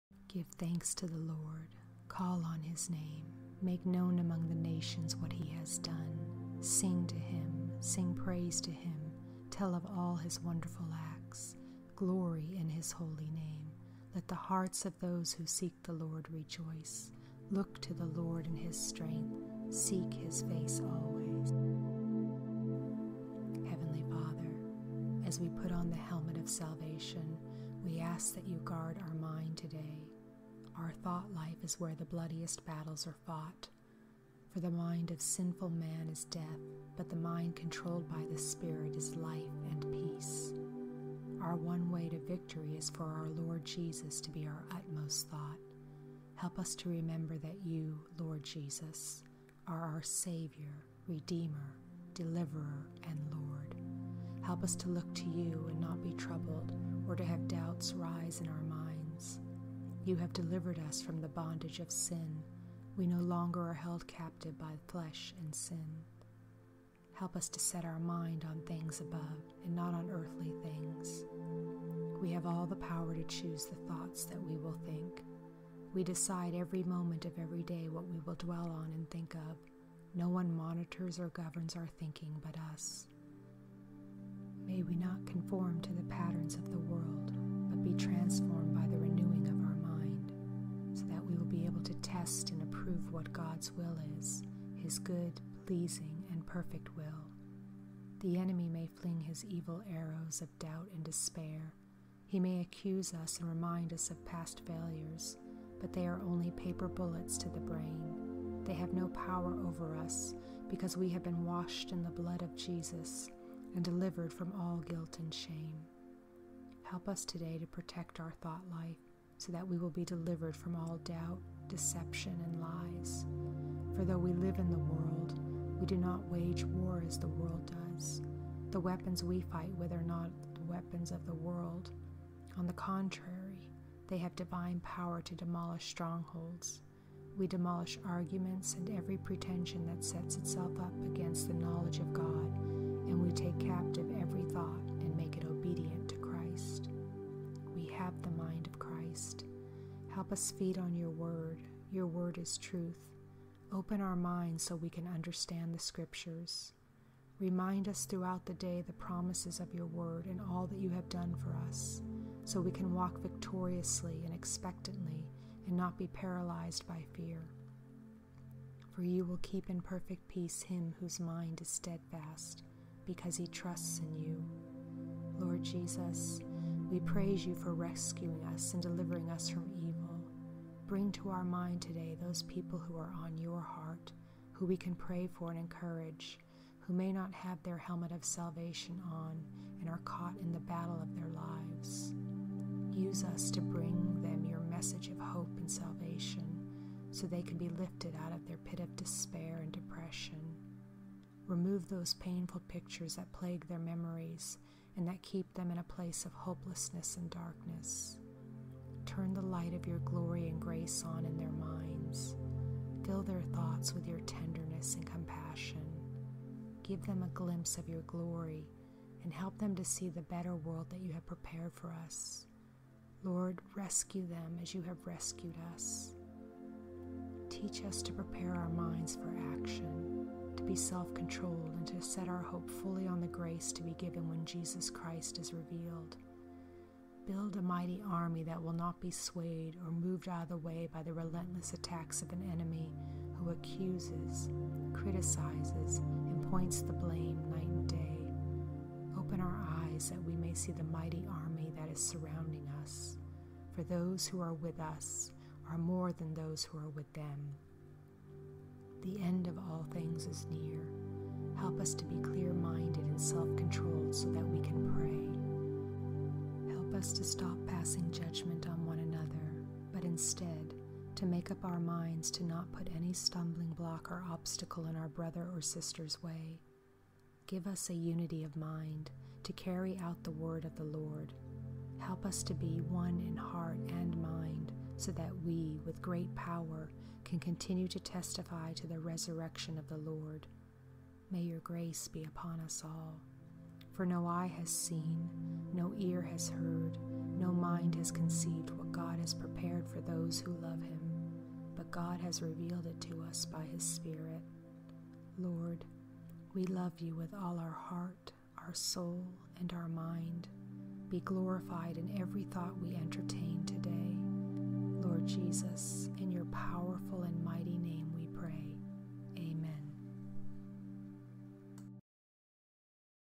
Prayer for the HARVEST - 40 Days (Day 34)
Prayer to protect our thought-life with the helmet of salvation.  "We demolish arguments and every pretension that sets itself up against the knowledge of God, and we take captive every thought to make it obedient to Christ." (2 Corinthians 10:5)